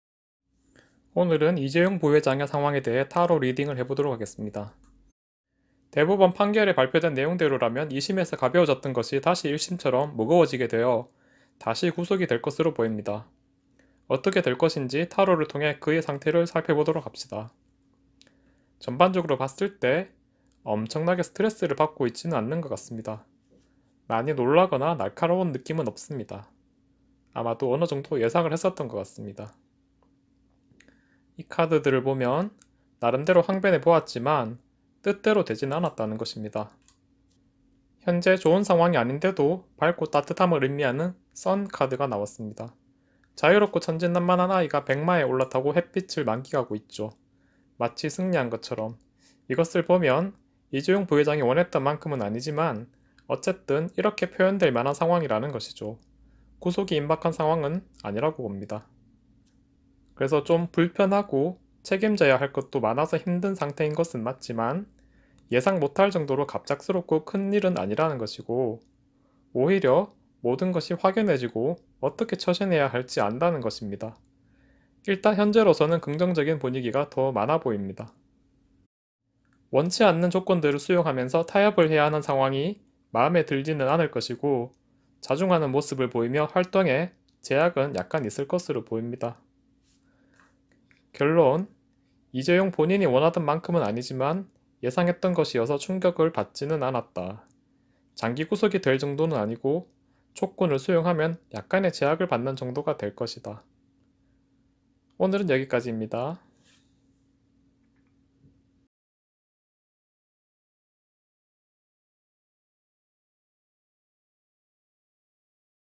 이재용 부회장 재수감 될 것인가? 안 될 것인가? [타로리딩/사주운세]
이번 대법원 판결에 의하면 2심에서 가벼워졌던 것이 다시 1심처럼 무거워져서 재구속이 될 가능성이 높아졌습니다.
과연 어떤 상태인지, 그리고 어떻게 될 것인지 타로리딩을 통해서 추측해 봅시다.

사주풀이는 개인의 운세의 전반적인 흐름을 볼 때 더 유용하고, 이번 일처럼 개인의 운세가 아니라 더 큰 흐름에 의해 영향을 받아서 발생한 사건에 대해서는 타로리딩이 필요합니다.

이메일로 사연을 보내 주시면 풀이해 드리겠습니다.
(태어나신 년월일시와 장소,  성별, 살아오면서 겪었던 일들, 질문을 적어 주세요.)